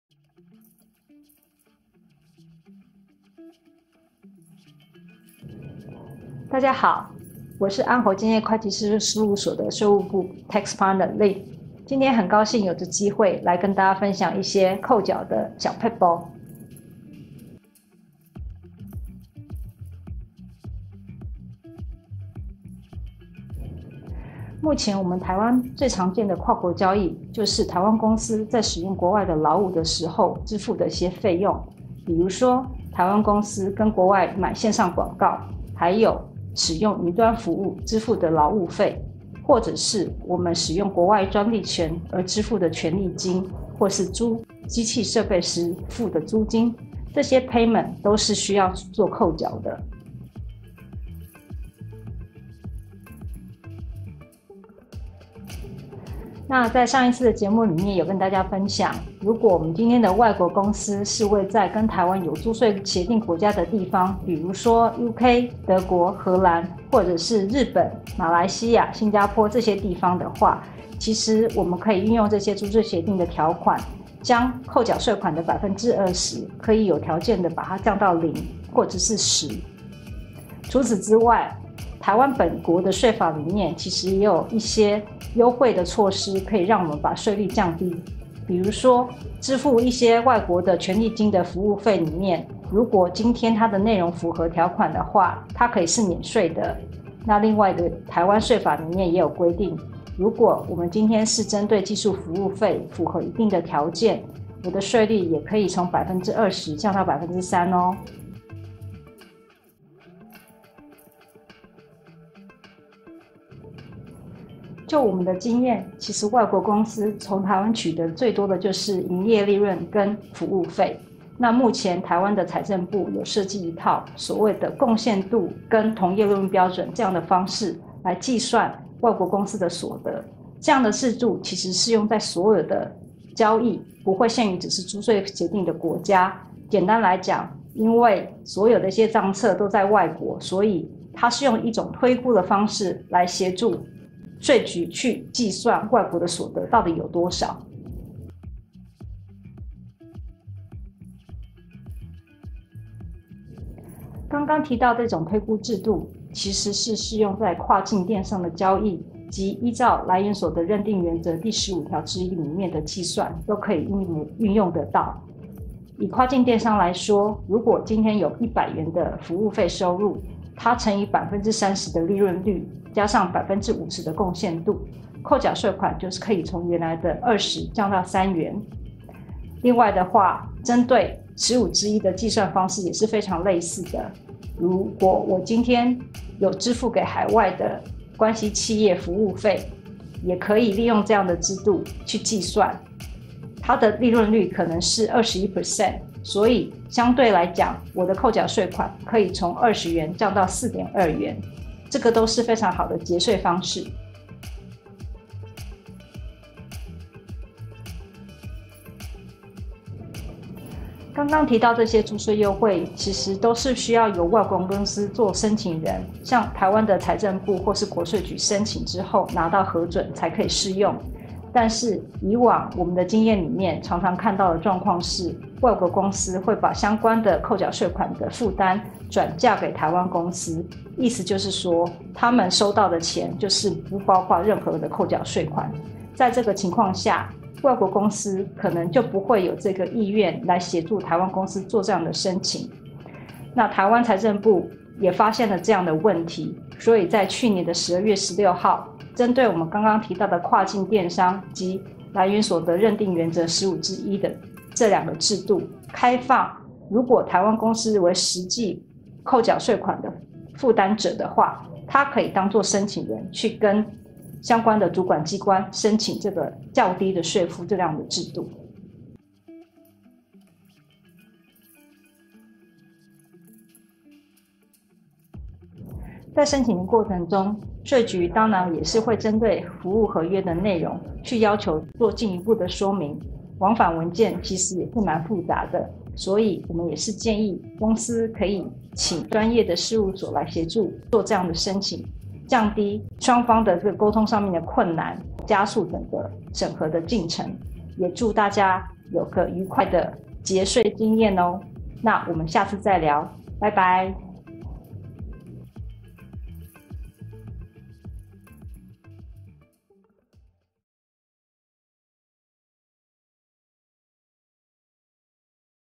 報稅不可不知！給付跨國技術服務，專家教你3招節稅！
企業購買跨國技術服務，如何降低應負擔之扣繳稅款：
（1）有租稅協定可適用
（2）申請所得稅法第25條關於技術服務規定
（3）申請淨利率及境內利潤貢獻程度

— 申請「淨利率」及「境內利潤貢獻程度」租稅優惠，適用範圍為何？扣繳稅款該如何計算？
— 實務上辦理稅捐減免應由國外或是台灣廠商提出申請？
— 當扣繳稅款由台灣企業承擔時，可採取哪些節稅措施？申請相關減免須具備哪些條件？

KPMG安侯建業稅務投資部執行副總經理 陳慧玲為您詳細解答。

訂閱KPMG in Taiwan頻道，最新消息不錯過 👇🏻👇🏻